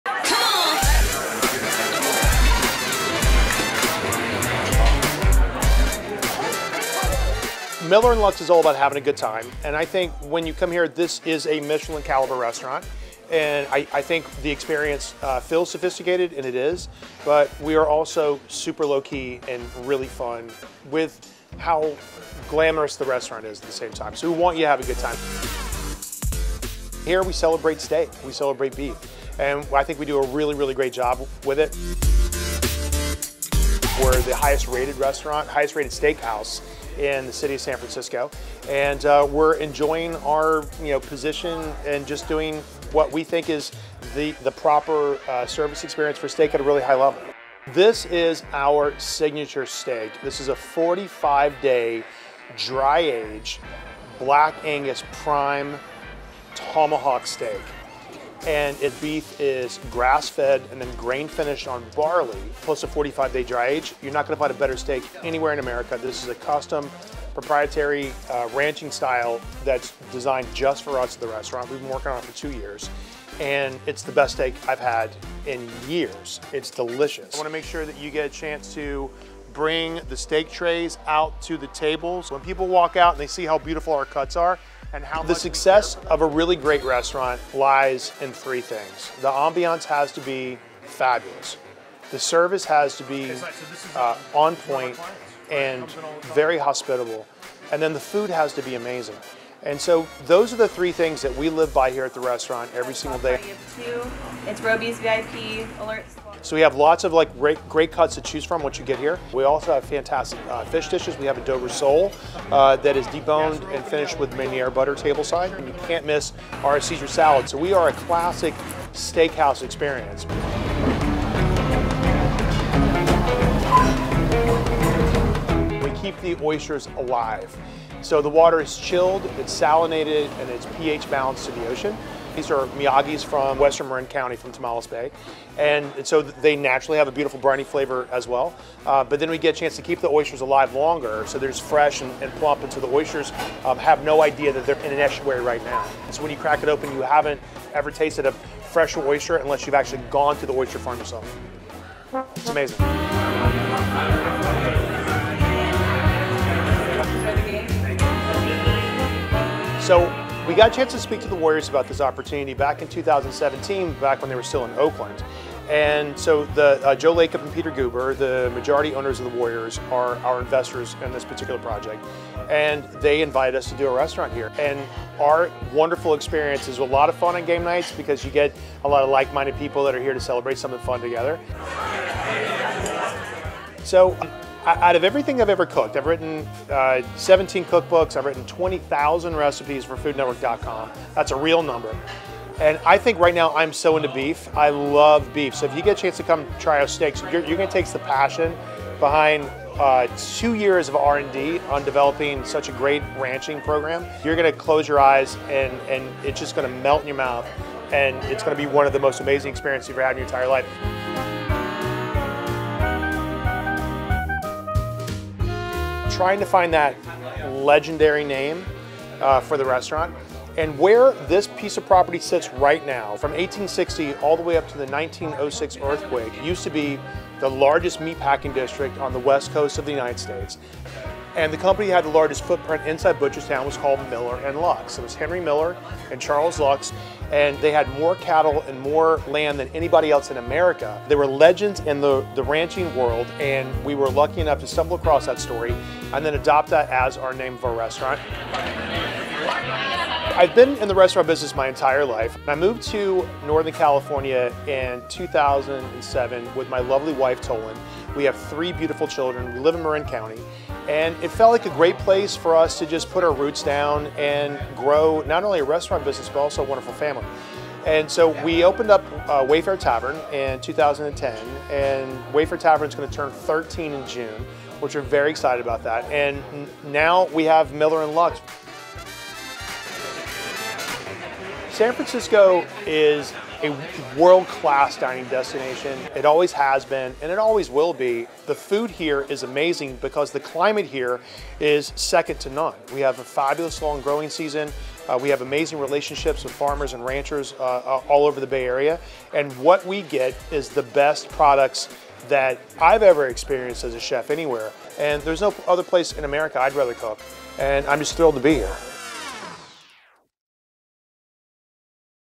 Take a seat at celebrity chef Tyler Florence’s steakhouse spot in SF
Celebrity chef and Food Network star Tyler Florence invite you to dine in his new refined and comfortable steakhouse,  Miller & Lux.

Located in San Francisco’s Chase Center, the popular spot is bursting with sophistication. On the menu, you’ll find signature steaks that are a cut above the rest.

"Miller & Lux is all about having a great time," said Florence. "I think the experience feels sophisticated, and it is, we are also super low-key and really fun with how glamorous the restaurant is at the same time."

He added, "Here we celebrate steak, we celebrate beef.

The menu is replete with grass-fed, aged, succulent options.

"This is a 45-day, dry-aged, Black Angus prime Tomahawk steak," said Florence of the restaurant’s signature steak. "The beef is grass-fed and then grain finished on barley, plus a 45-day dry age, you're not going to get a better steak anywhere in America."

While the steaks may be fan favorites, the oysters are also a popular choice. They’re kept in an estuary-like habitat at the restaurant for ultimate freshness.

"We keep the oysters alive. So, the water is chilled, it's​​ salinated, and it's pH balanced to the ocean," described Florence. "Then, we get a chance to keep the oysters alive longer, so they’re fresh…when you crack it open, you haven’t tasted a fresher oyster unless you’ve actually gone to the oyster farm yourself."

The restaurant has a secret door that leads into the Chase Center arena.

"We got a chance to speak with the Warriors about this opportunity back in 2017," expressed Florence. "Our wonderful experience is a lot of fun on game nights because you get a lot of like-minded people who are here to celebrate some of the fun together."

Florence’s passion for the Bay Area is reflected in the new spot.

"I've been in the restaurant business my entire life. I'm 52 years old now, and this is my 27th year on television with Food Network, I moved to Northern California in 2007 with my lovely wife Tolan. We have three beautiful children," said Florence. "We live in Marin County, and it felt like a great place for us to just put our roots down and grow not only a restaurant business but also a wonderful family. And now we have Miller & Lux!"